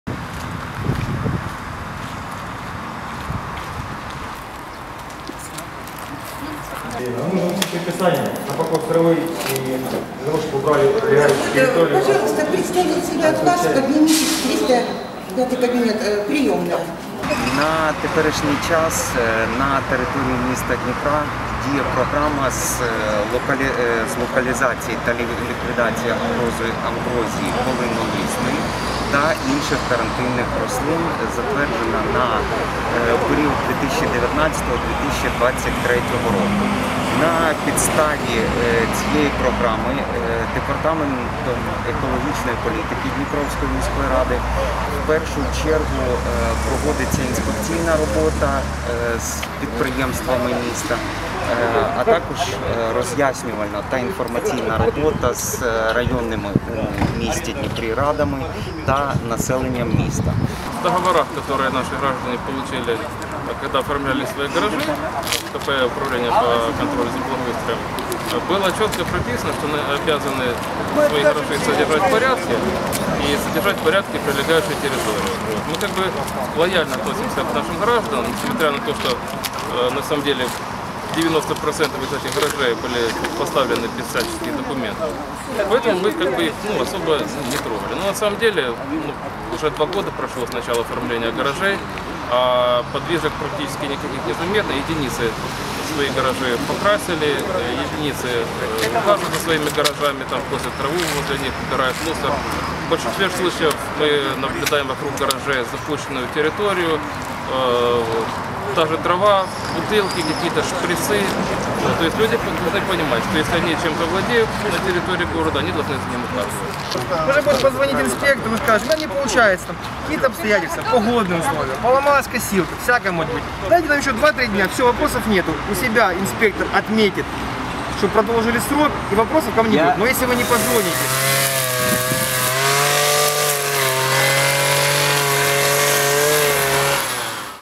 Інспекційний рейд з локалізації та ліквідації амброзії та інших карантинних рослин у Дніпрі
У Дніпрі проводиться комплексна робота з локалізації та ліквідації амброзії та інших карантинних рослин у межах відповідної муніципальної програми. Вона включає низку заходів, серед яких, зокрема, й роз’яснювальна та інспекційна робота щодо утримання прилеглих територій у належному санітарному стані. Йдеться передусім про вчасне скошування трави, карантинних рослин, викорчовування амброзії. Це стосується як підприємств та установ усіх форм власності, так і мешканців приватного сектору.

Черговий інспекційний рейд 18 червня спільно провели департамент екологічної політики міської ради та інспекція з питань благоустрою міськради.

«Головне наше завдання – не оштрафувати одразу за непокошену траву, а донести до людей, що прилегла до будівель територія має бути прибраною як від сміття, так і високої трави та бур’яну. Спочатку виписуємо припис, а якщо їх не усувають, складаємо адміністративні протоколи щодо порушення правил благоустрою міста. Мешканці самі повинні розуміти, як негативно та ж амброзія впливає на здоров’я», – розповів начальник відділу екологічної безпеки департаменту екологічної політики ДМР Ярослав Крисько.

Під час перевірки прилеглої території на вул. Журналістів, 7 виявили непокошену траву та карантинні рослини, а також будівельне сміття. Інспектори оформили припис на керівництво установи і дали п’ять днів на усунення порушень. Якщо цього не буде зроблено, інспекція складе адміністративний протокол.

«За останні два місці інспектори провели понад двадцять рейдів щодо вчасного скошування трави на прилеглих територіях. На порушників виписано близько 50 приписів, складено понад 20 адмінпротоколів. Роботу не зупиняємо, тобто і надалі інспектори проводитимуть такі рейди. Тому закликаємо мешканців, керівників підприємств бути сумлінними, стежити за прилеглою територією, щоб там була покошена трава, не було сміття, бур’янів і карантинних рослин», – наголосив начальник інспекції з питань благоустрою ДМР Руслан Мороз.

Нагадаємо, утримання прилеглої території у неналежному санітарному стані – це порушення правил благоустрою. Розмір штрафу для фізичних осіб становить до 1360 грн, для юридичних – до 1700.